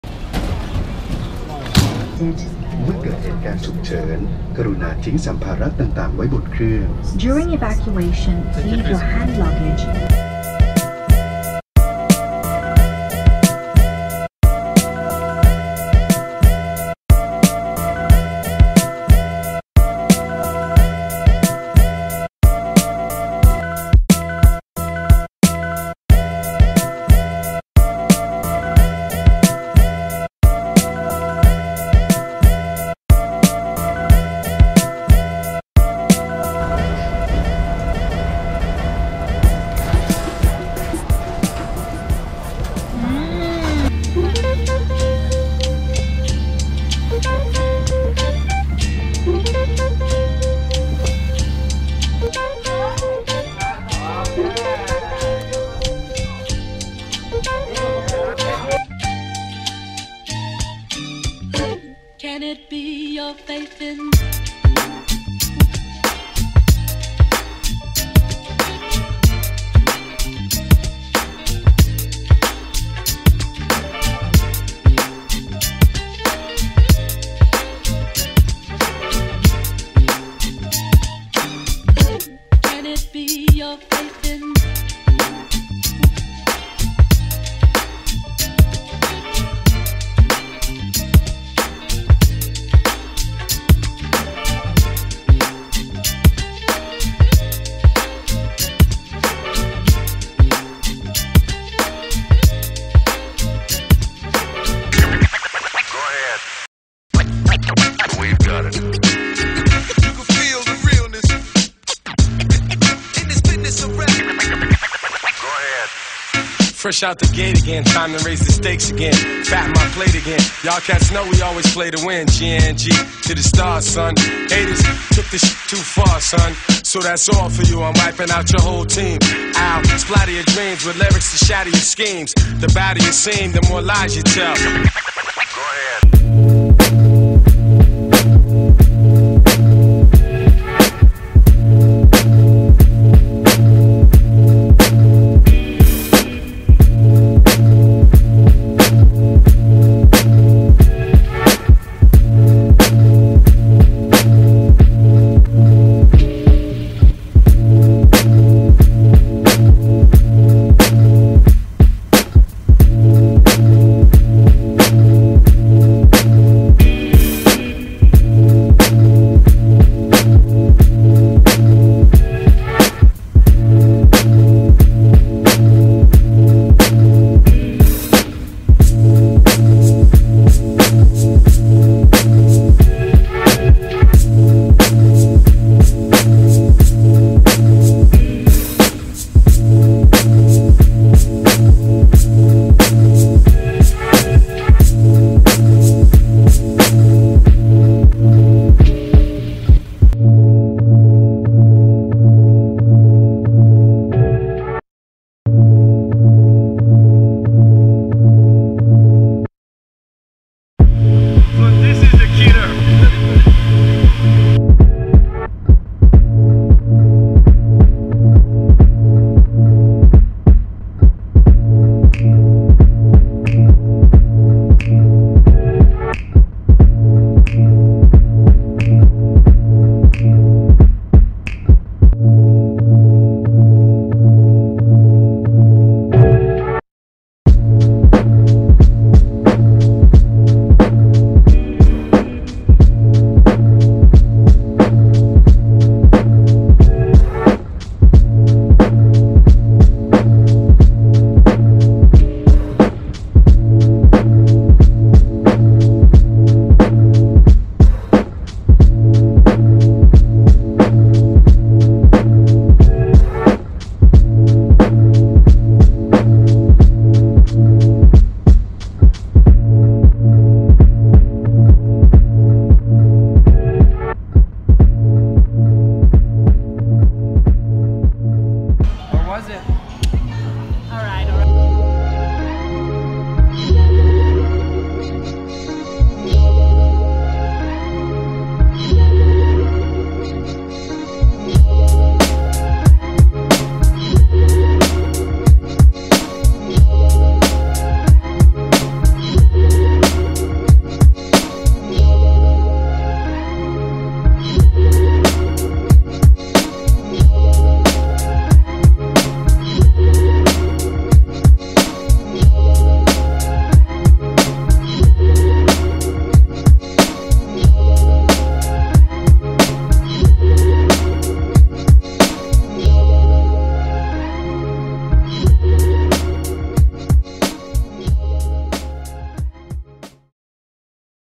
South East Asia Trip #3 - Thailand
The last chapter of our road trip in Indonesia and South East Asia.
Islands, scuba diving, rock climbing, tanning, sailing, night markets, beaches,.. 3 amazing months backpacking!